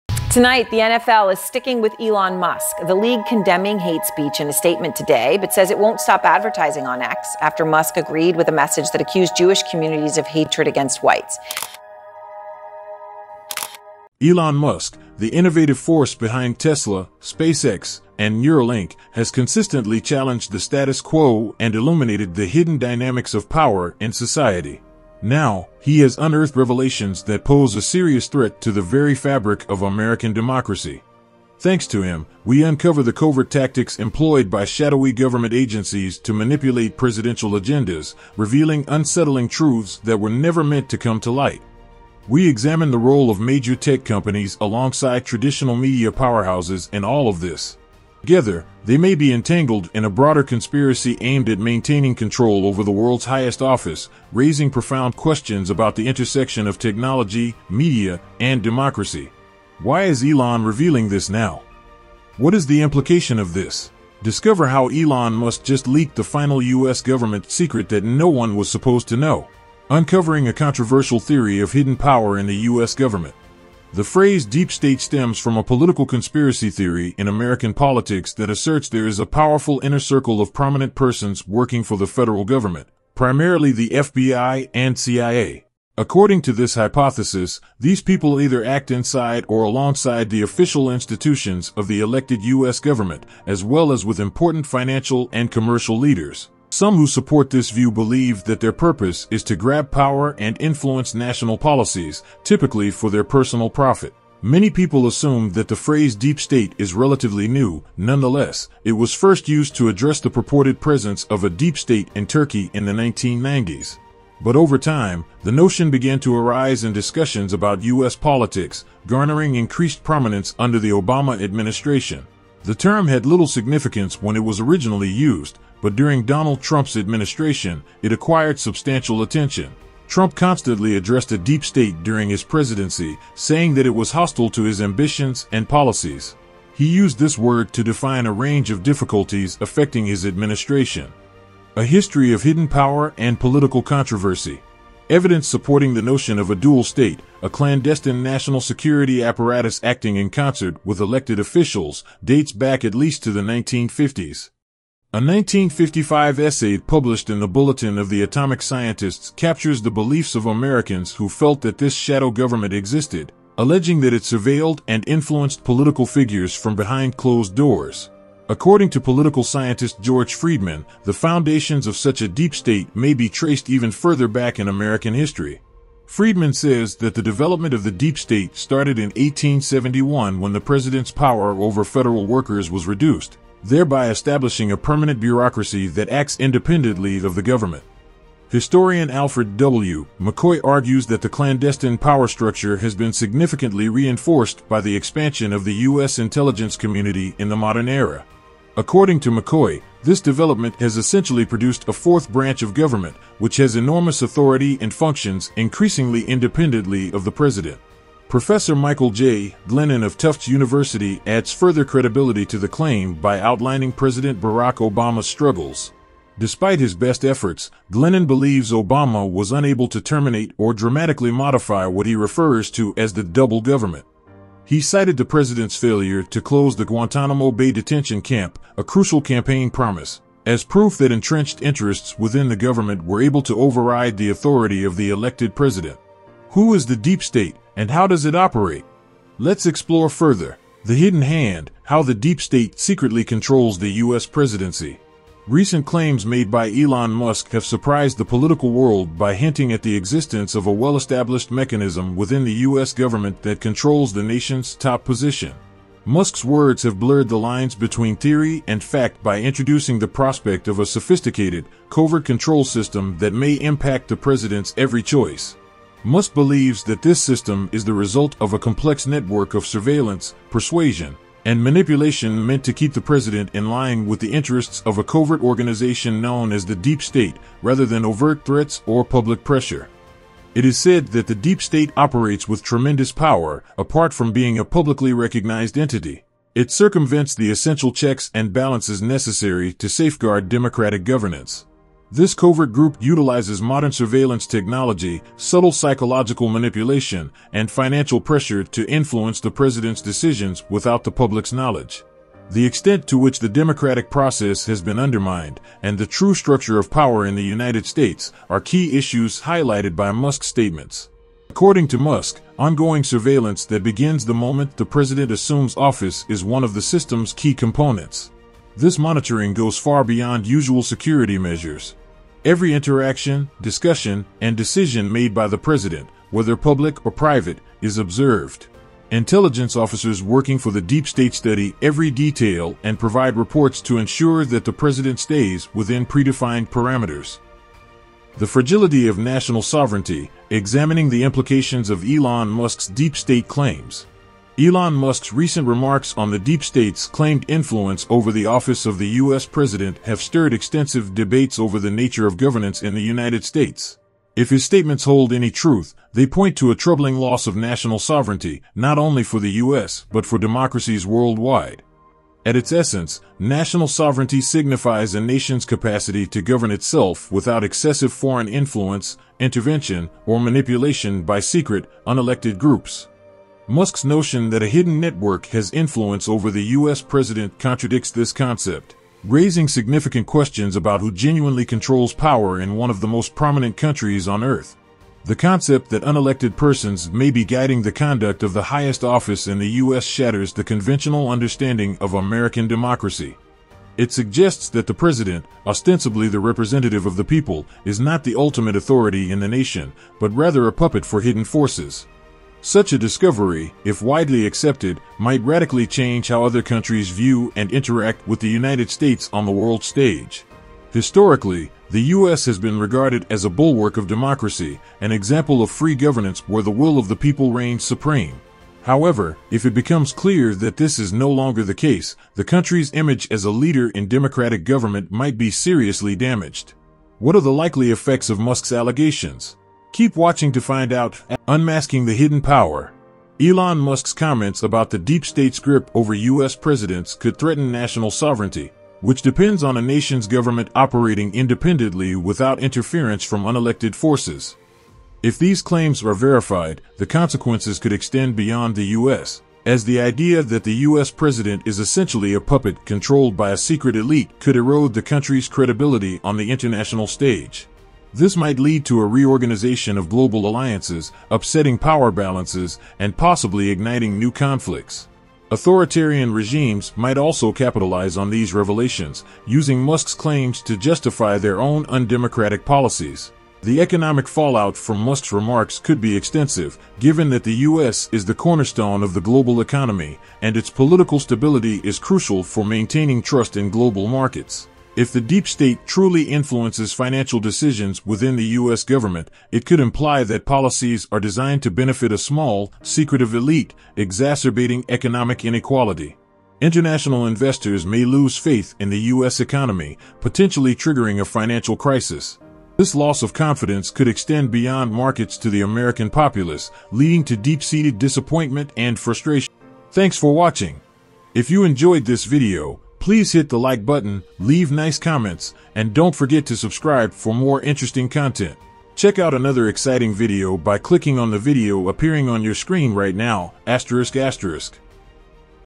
Elon Musk Just LEAKED The Final US Government Secret That No One Was Supposed To Know!
🚨 Elon Musk's Shocking Revelations: The Deep State's Hidden Power Over U.S. Presidents 👀

Elon Musk, the visionary behind Tesla, SpaceX, and Neuralink, has dropped another bombshell 💣! This time, it's not about rockets or AI—it's about the very fabric of American democracy 🗽. Could the U.S. President really be controlled by secret, unelected forces? 🤯 Musk suggests YES! In this video, we dive deep into his claims, uncovering the shadowy tactics of hidden government agencies 👥, tech giants 📲, and media powerhouses 📺. Are they pulling the strings behind the scenes?

🔍 Is this the ultimate conspiracy theory or a hidden truth finally exposed? And why is Musk revealing this NOW? 🤔

🌐 What You'll Discover:
- The chilling reality of a possible Deep State at work 🕵️
- How tech companies and media giants might be entangled in this web 🖥️
- What this means for U.S. sovereignty 🇺🇸 and democracy worldwide 🌍
- Economic and political ripples that could change everything 🌪️

🚀 Get ready to have your mind blown as we explore this hidden power! Watch till the end to find out the jaw-dropping implications of Musk's revelations.

📺 Next up: Check out another mind-bending video by clicking on the screen right now! You won’t want to miss it! 🎬

TIMESTAMPS⏱️:
00:14 Intro
01:12 Number 1
02:21 Number 2
04:01 Number 3
05:38 Number 4
07:12 Number 5